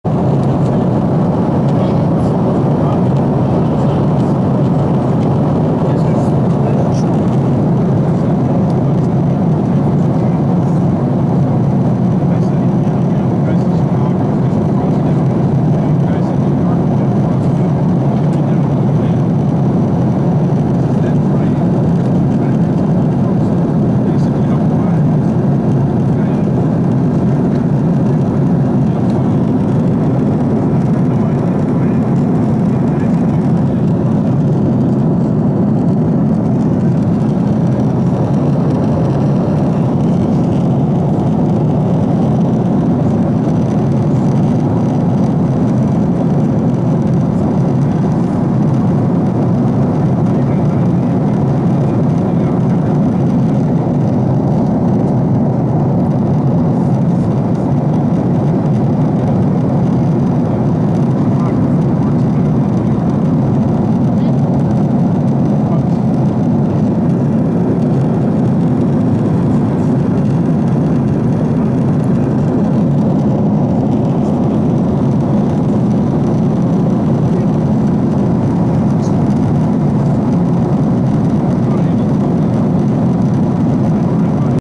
UNITED FLYING OVER THE ALPS IN ROUTE NEW YORK - MILAN
United Boeing 767-400 flying over the alps in route EWR-MXP